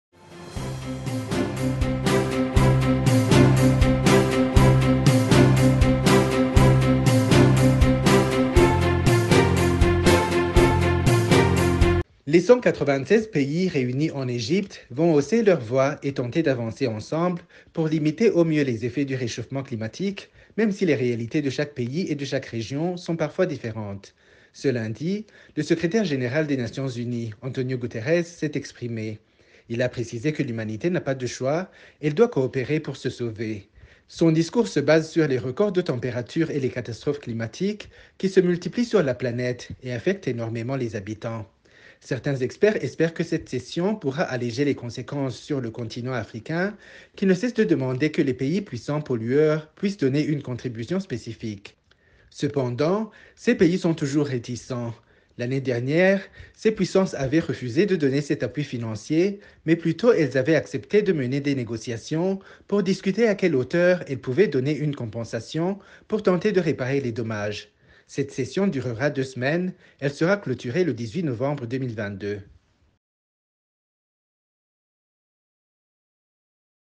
COP27 qui se tient en Egypte, espoir pour remédier au changement climatique
C’est ce dimanche qu’a commencé la conférence mondiale des Nations Unies (COP27) sur le climat, à Sharm el-Sheikh en Egypte. Cette session qui se tient tous les 6 ans a pour objet d’étudier et d’analyser les effets du réchauffement climatique, son impact sous toutes ses formes. Près de 200 pays, dont le Burundi, prennent part à ce sommet. Cette conférence est la première à se tenir sur le continent africain.